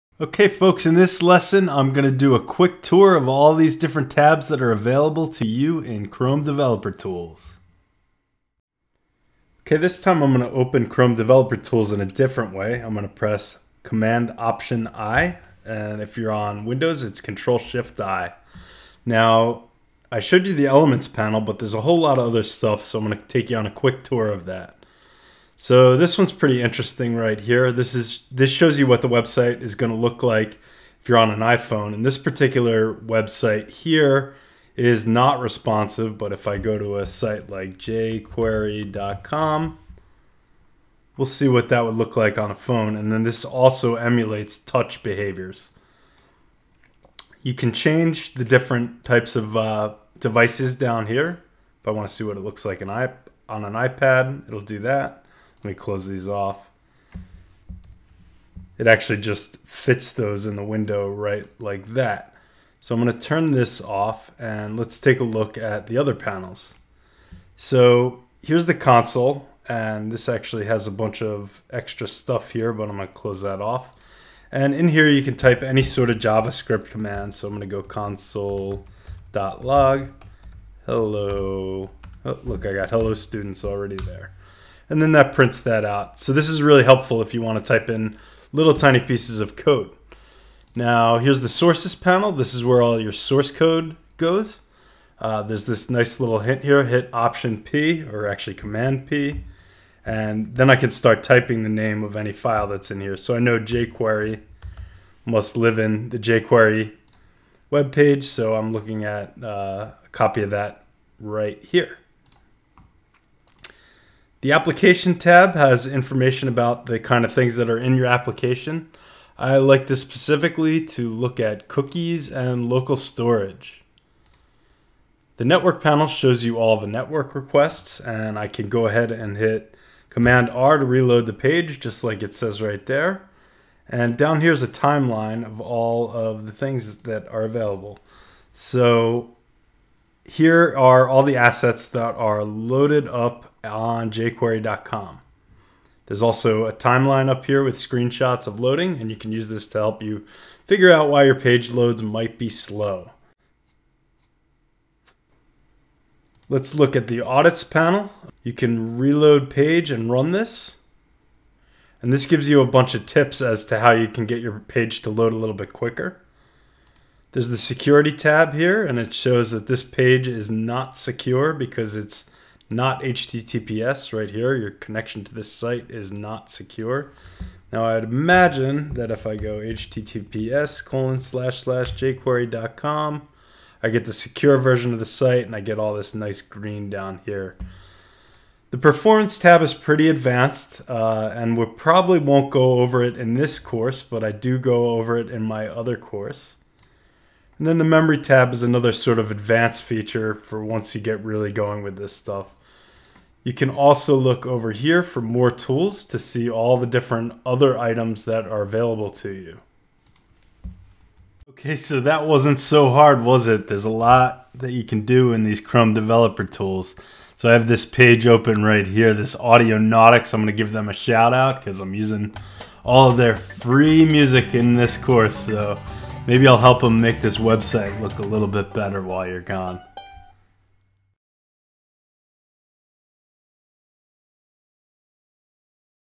Chrome DevTools Tutorial 4: A Tour of the Developer Tools
Part X of a Google Chrome Developer Tools tutorial.

Run through each of the tabs in DevTools and understand the basics about what each of them does. The panels shown are: Elements, Console, Sources, Application, Network, Performance / Timeline, and Memory